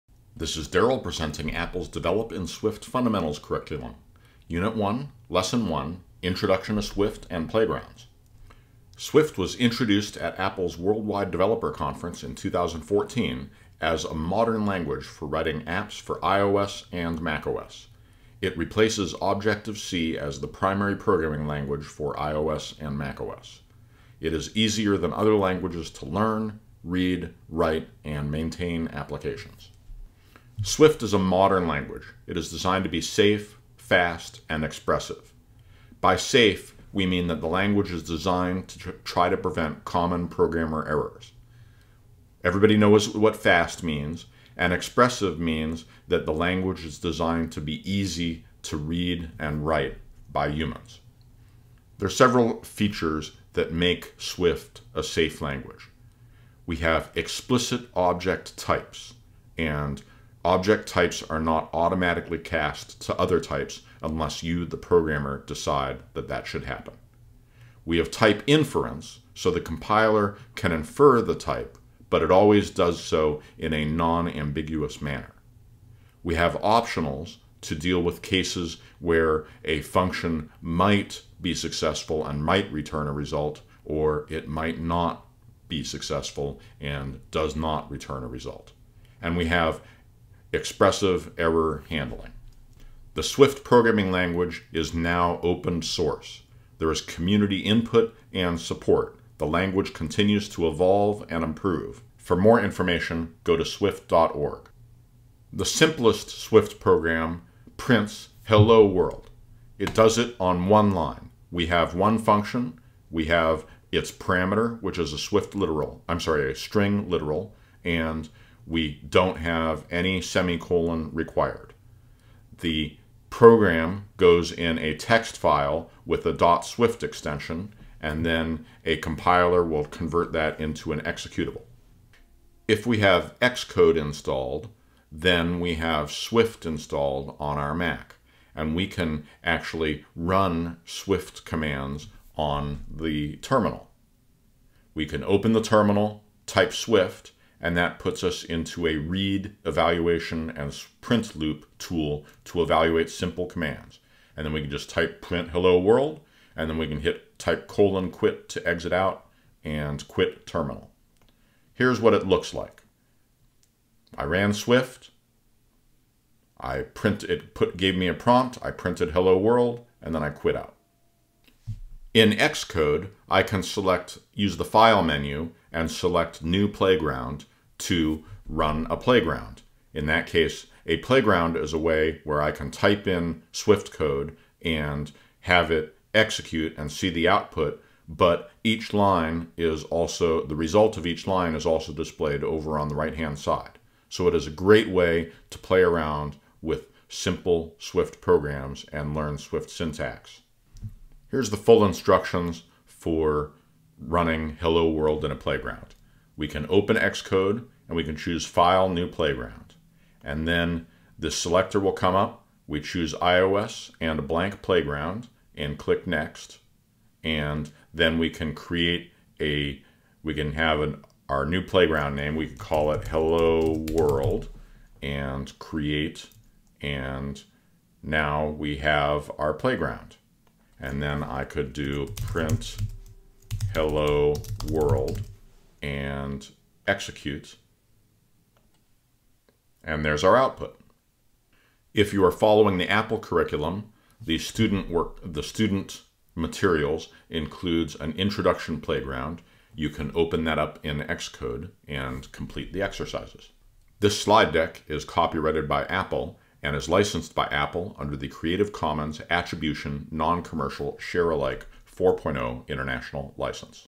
Swift Programming Fundamentals Unit 1 Lesson 1: Introduction to Swift and Playgrounds
Apple Education wrote a "Develop in Swift Fundamentals: Xcode 13" curriculum, including slide decks.  Darrell presents the slide deck from unit 1 lesson 1 which is a 4-minute presentation introducing Swift and Xcode Playgrounds.

The student book is free from Apple  The book includes links to download "student materials" including the labs.

The teacher guide is free from Apple It includes links to download "teacher materials" including solved labs and the slide decks.